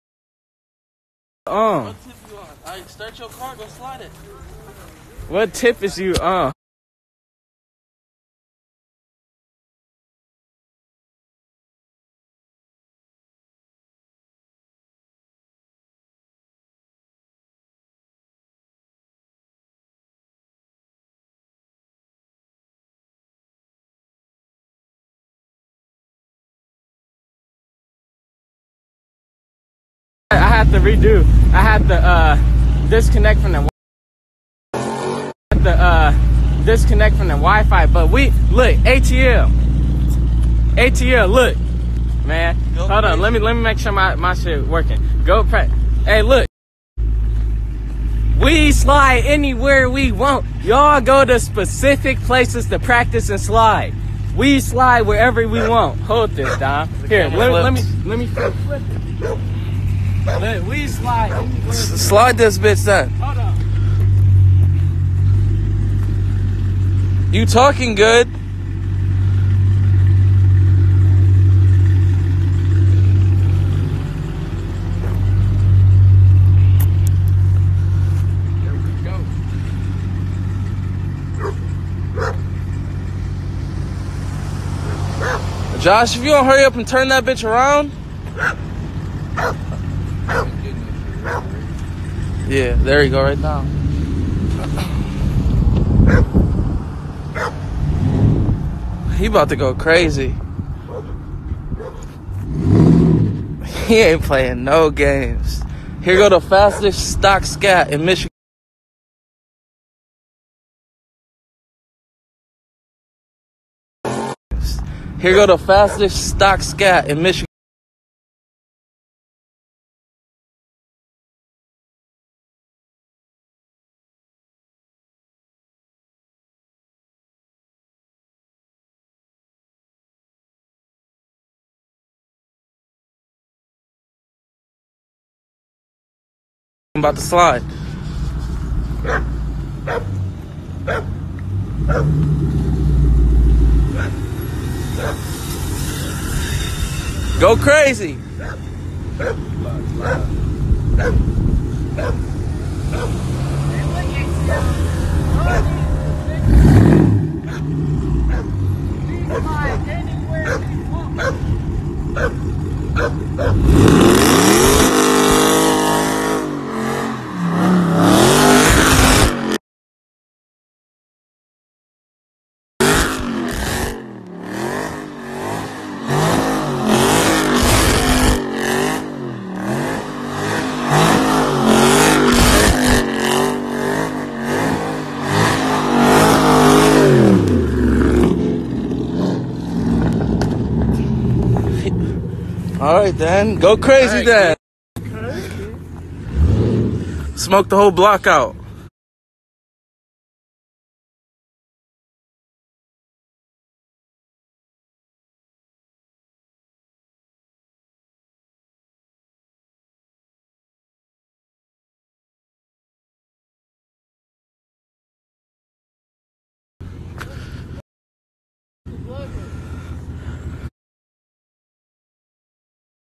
Josh Laurent Slides the Fastest Stock Scat in Detroit on Live after being told to Practice✊🏽🔥
This friendly beef been starting up some stuff, let’s see who comes out with a win in July🤣🙌🏽 Comment which side y’all got y’all money on!

*I do not take any ownership of music displayed in the video. Ownership belongs to the respected owner(s).